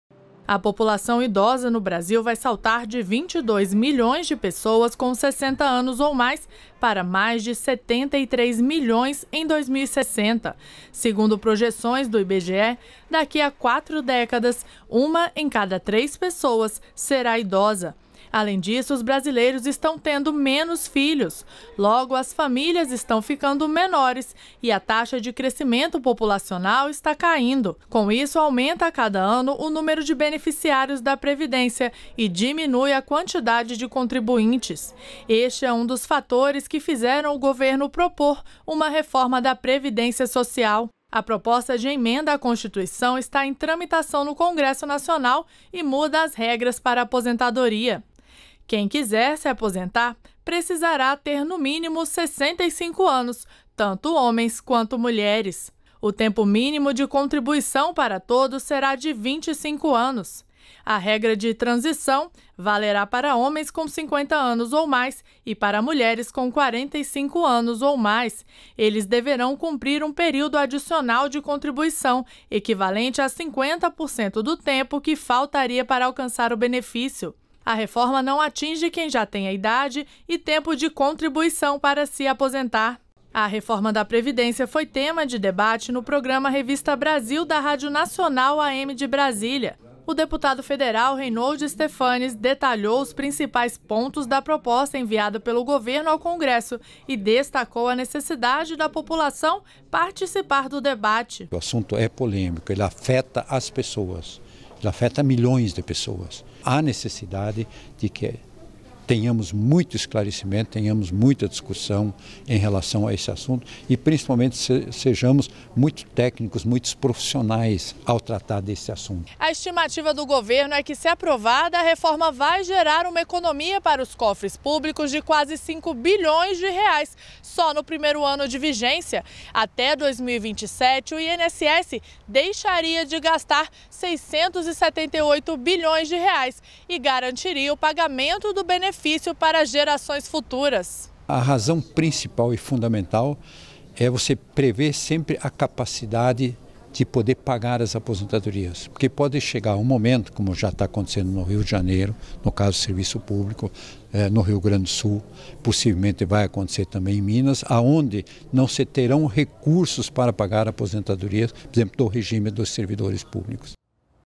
Rádio Nacional promove debate sobre a reforma da previdência
REPÓRTER NBR - 03.04.17: A Rádio Nacional promoveu mais um debate sobre a reforma da previdência social. Desta vez, o convidado foi o deputado federal  e ex-ministro da Previdência Social Reinhold Stephanes (PSD-PR).